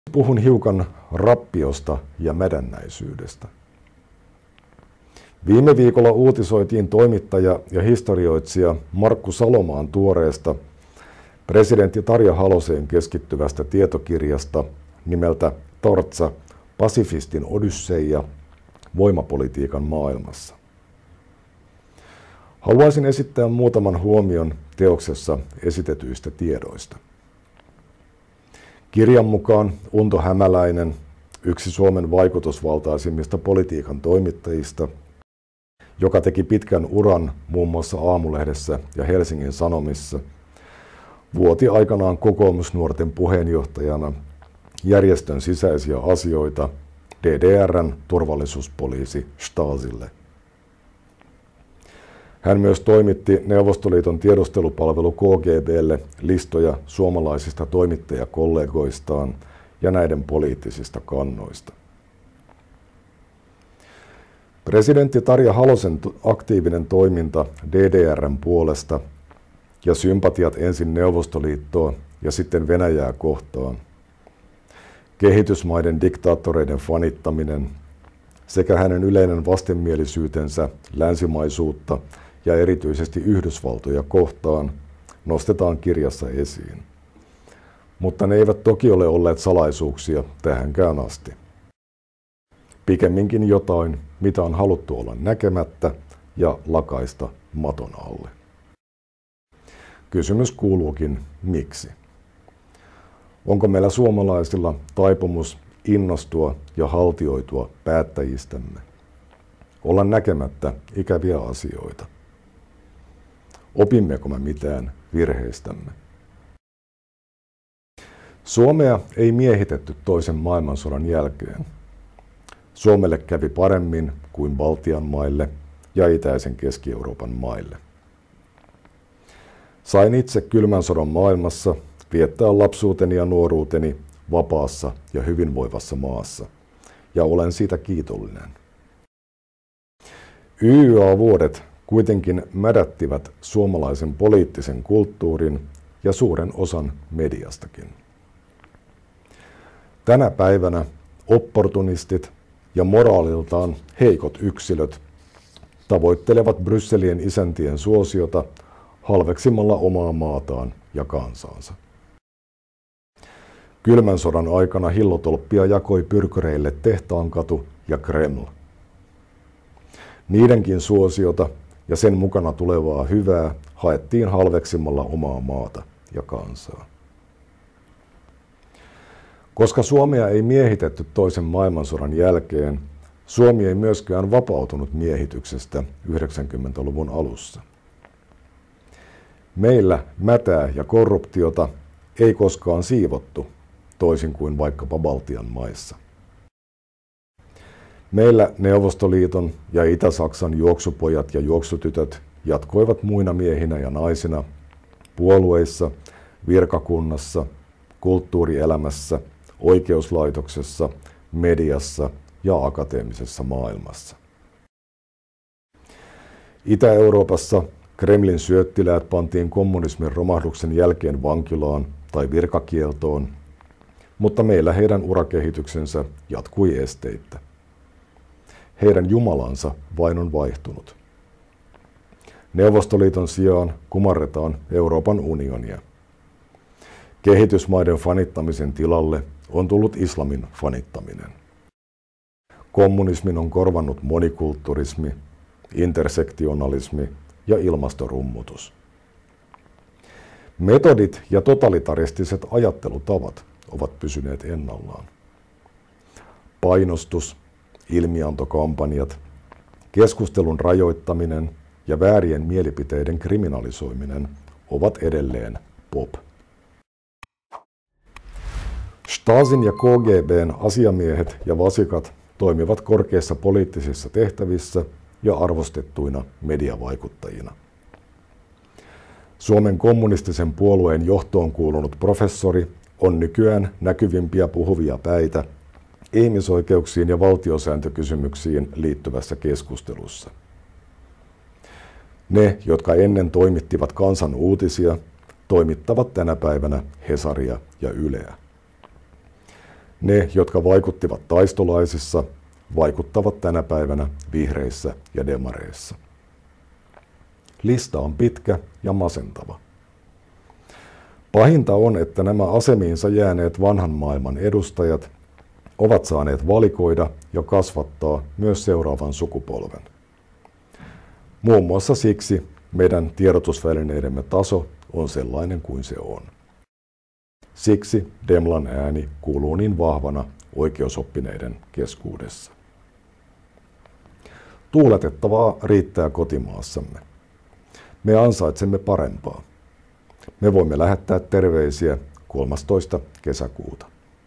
Stasin ja KGB:n vasikat toimivat Suomessa korkeissa poliittisissa tehtävissä ja mediavaikuttajina
Jussi Halla-aho puhuu vappupuhessaan Suomessa vallitsevasta rappiosta ja mädännäisyydestä.